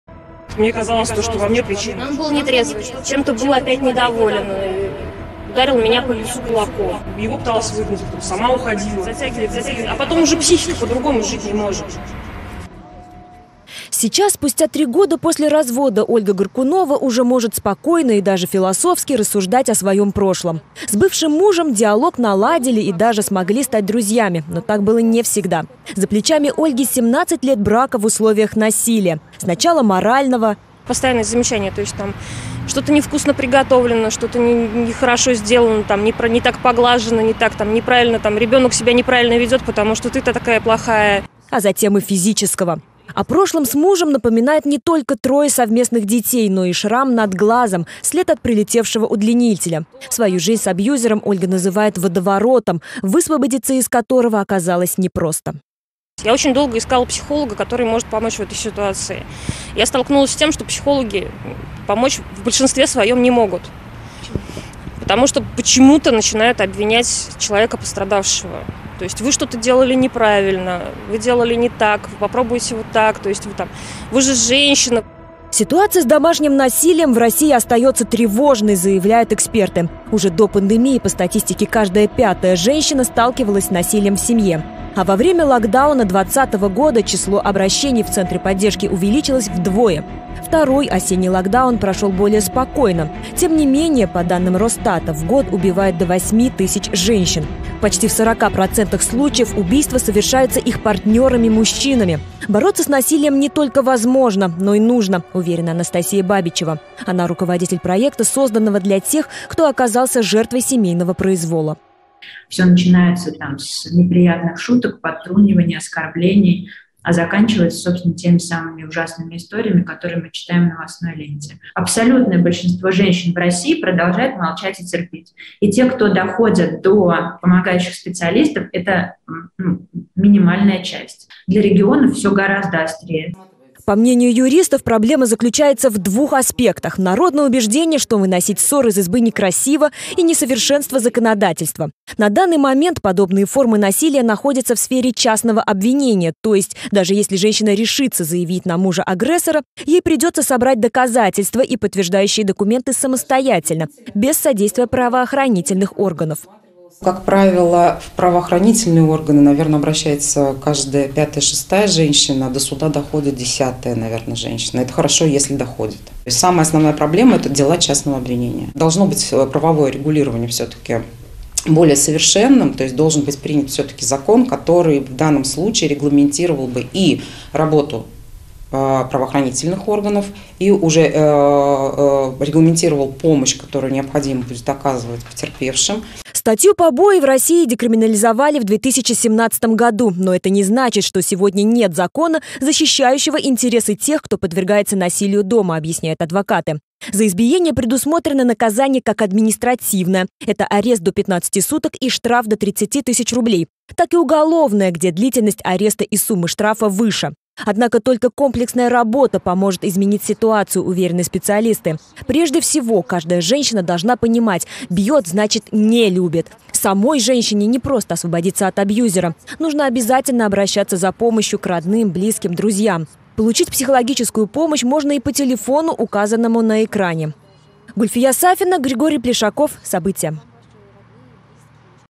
Ситуация с домашним насилием в России остается тревожной
По статистике, каждый локдаун приводит к росту случаев домашнего насилия. Весной 20-го года число обращений в кризисные центры и отделы полиции выросло в 2,5 раза. Каждая пятая женщина заявляет, что ее пытались убить. Гульфия Сафина пыталась разобраться, почему тема домашнего насилия по-прежнему остается острой, как на сегодняшний день жертвы агрессоров защищены законодательно и что делать, чтобы переломить устаревший стереотип - "бьет, значит любит".